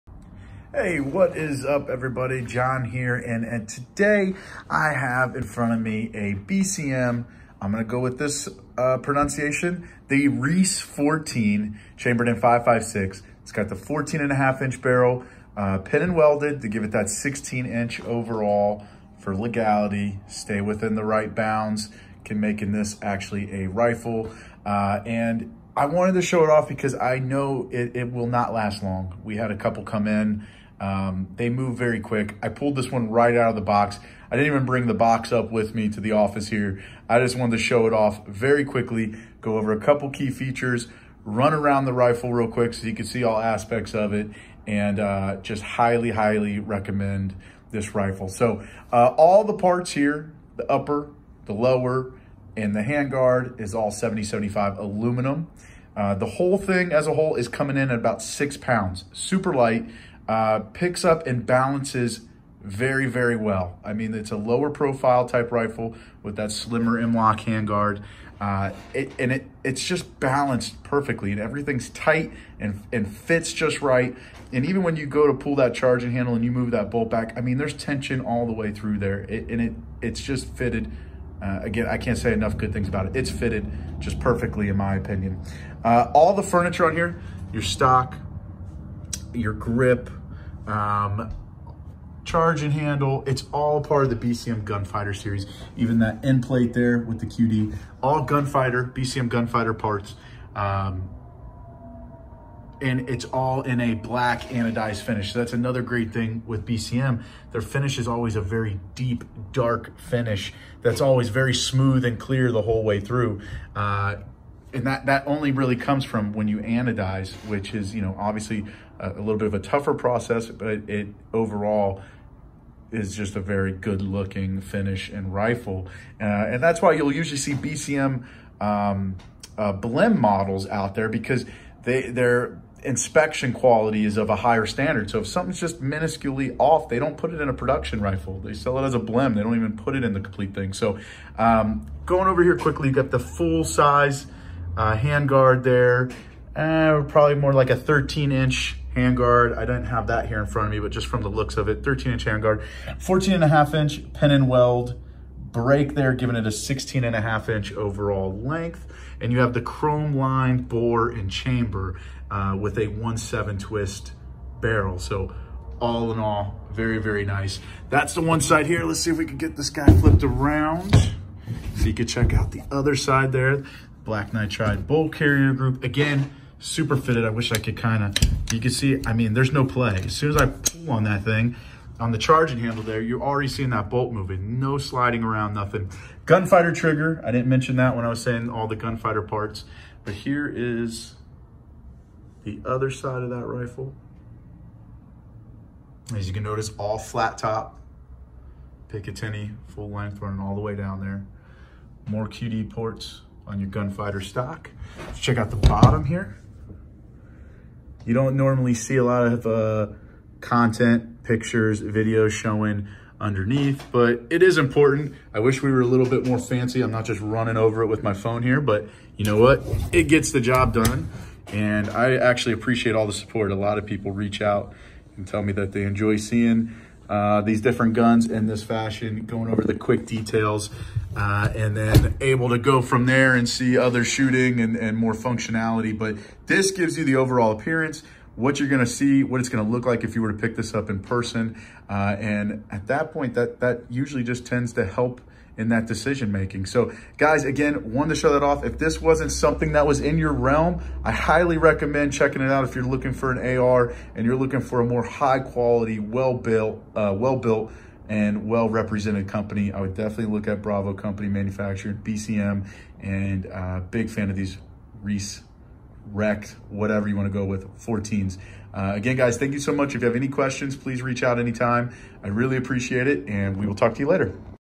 BCM Reece-14 14.5” (16”OAL) 5.56 AR-15 - Tabletop rundown
Key Features:
BCM
Model: Recce-14
14.5" barel
1.5" pin & welded brake (Overall 16")
Chromed Bore & Chamber
5.56 NATO Chamber
7075-Aluminum
Anodized finish
Gunfighter Trigger & furniture